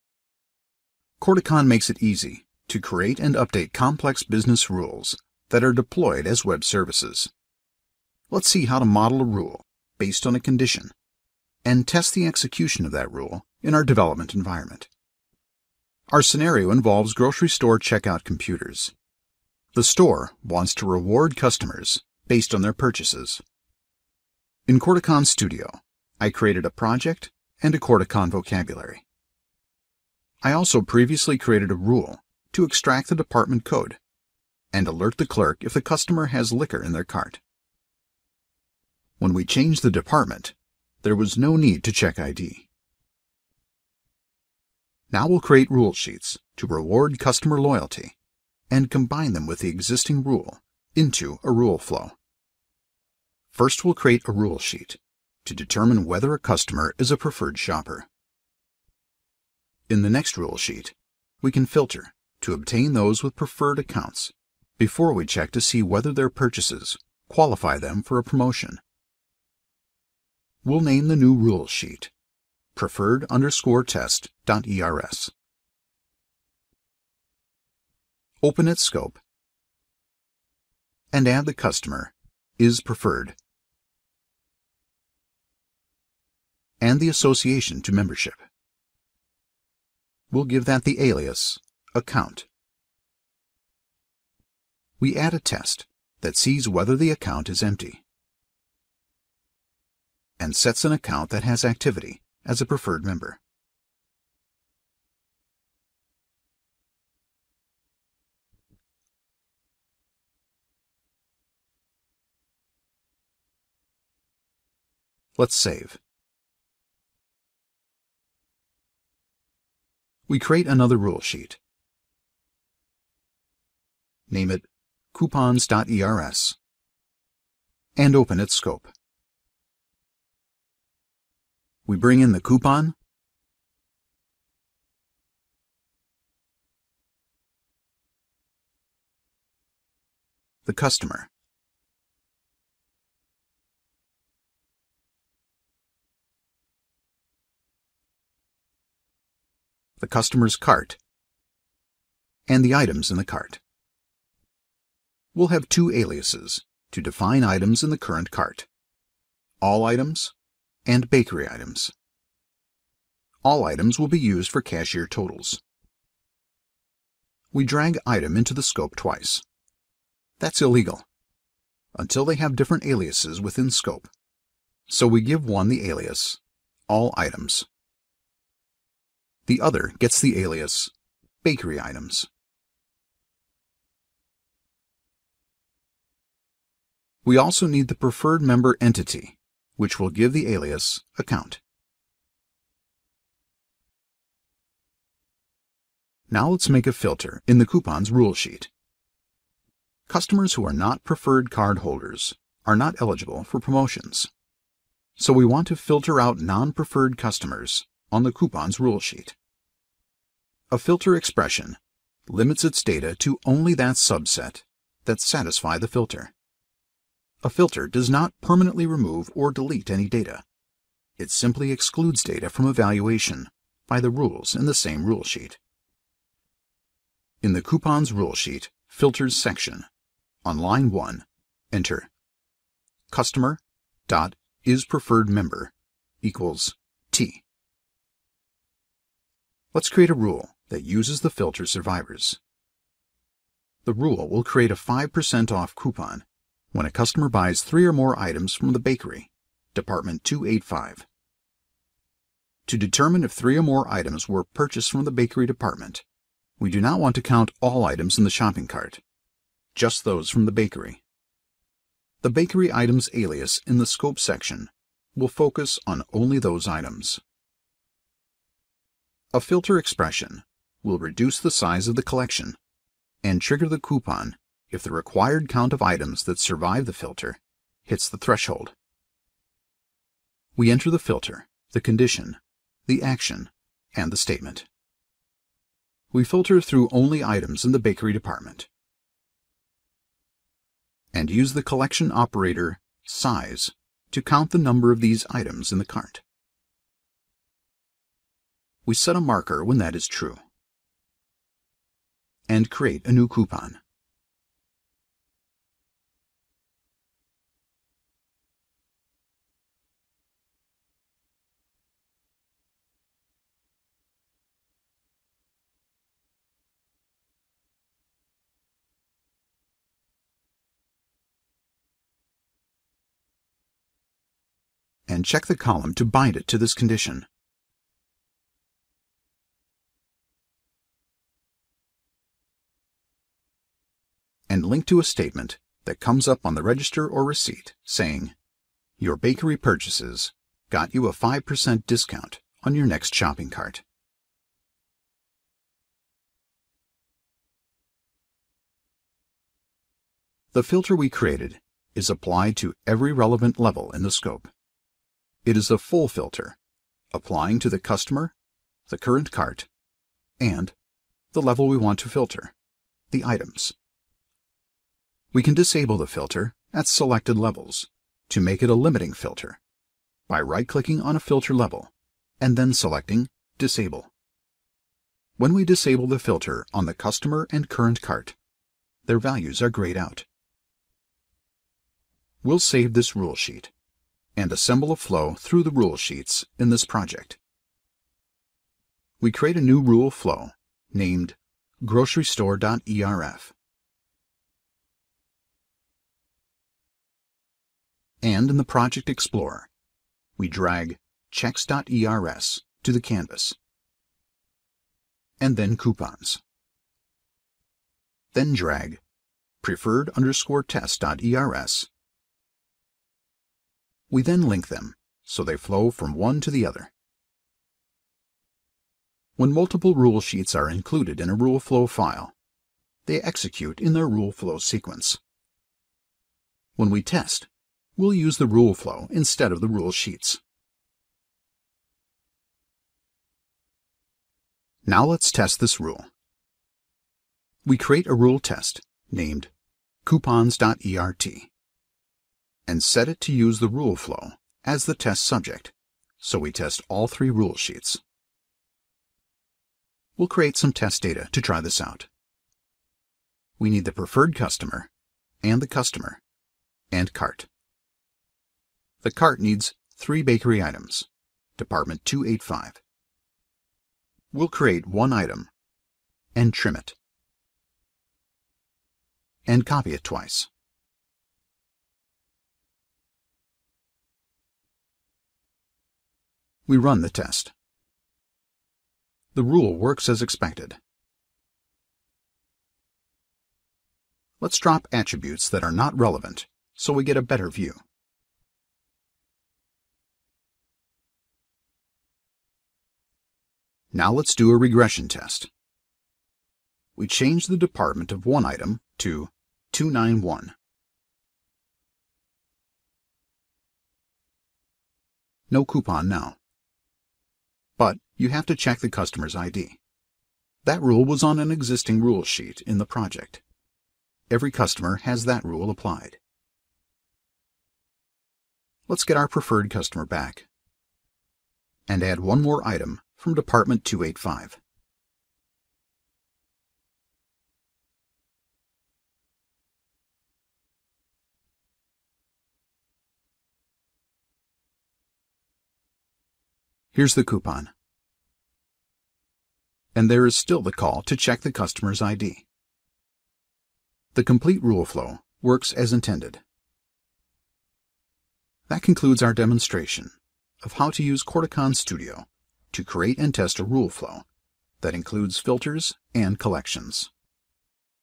Corticon A walk through the advanced tutorial
Corticon makes it easy to create and update complex business rules that are deployed as web services. In this video, you will see how to model a rule based on a condition and test the execution of that rule in our development environment.